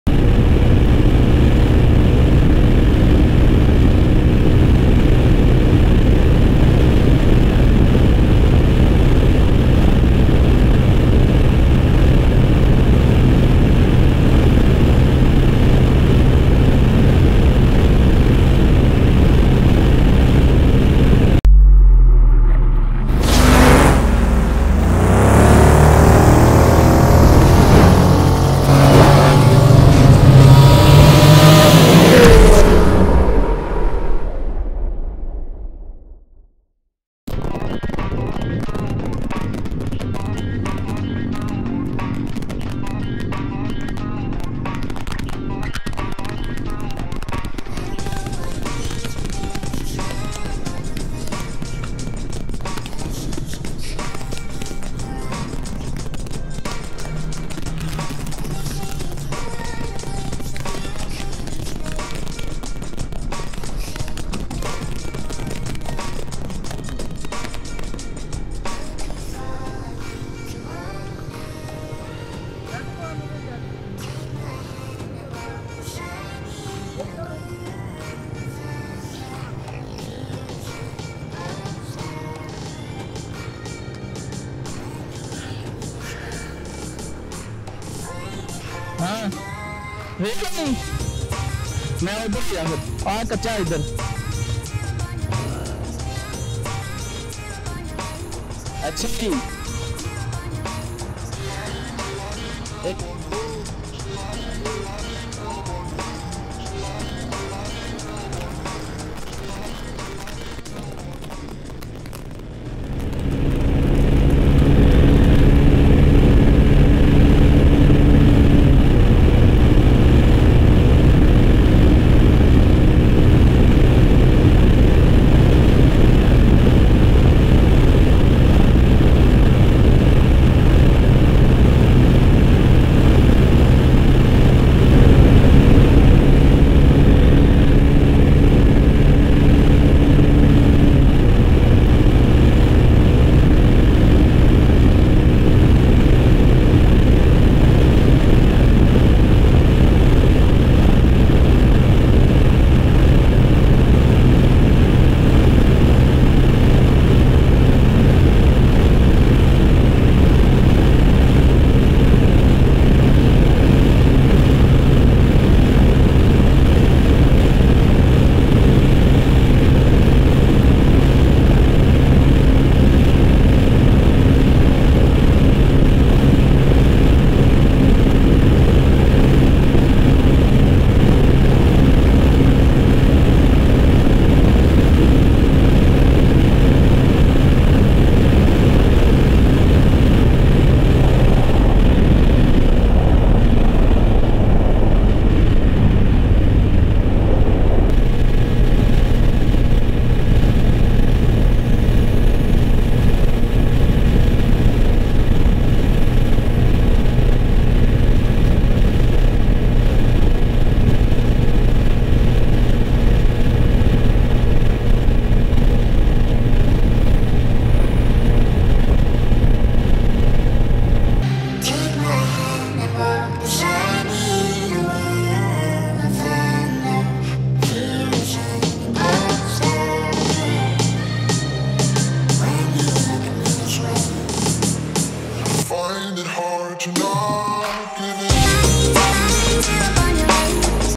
Honda CG125Se vs Yamaha YBR125G vs Yamaha YB12Z DX || Drag Race
Hello everyone and welcome back to channel.
Today's video is a DragRace Between hondavsYamaha Yb125Z-DX and YBR125G.

Watch the complete video. Share your opinion that how it looks.